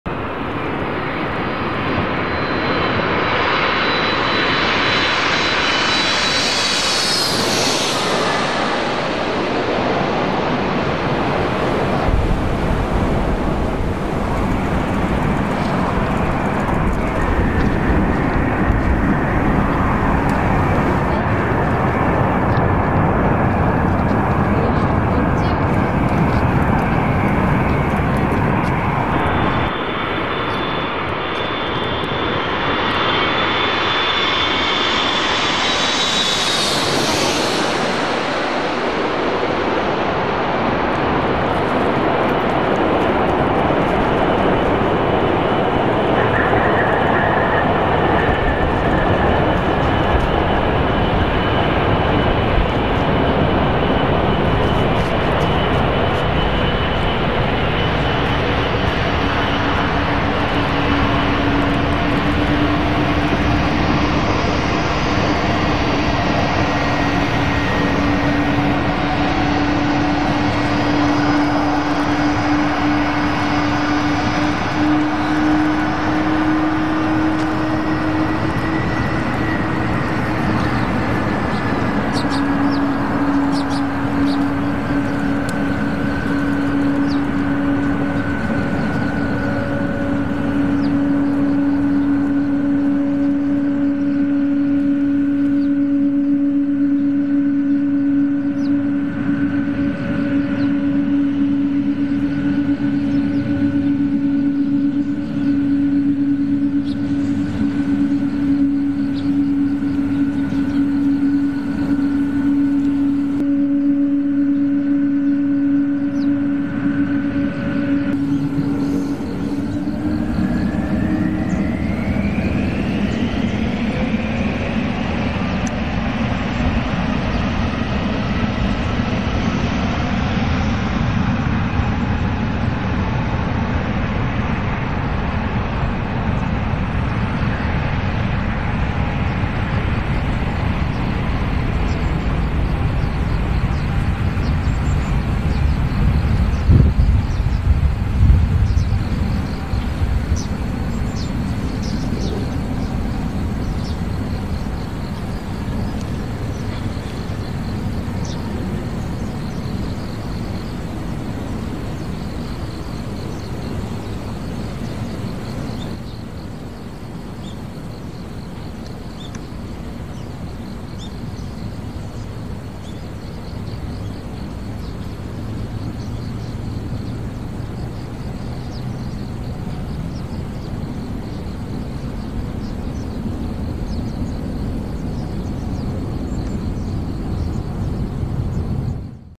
何度も訪れたい千里川の迫力 / 伊丹32L JAL 777-200 787-800
何回でも訪れてみたい伊丹の聖地、迫力と魅惑の千里川エンド。JAL 777-200・787-800型機の離着陸です。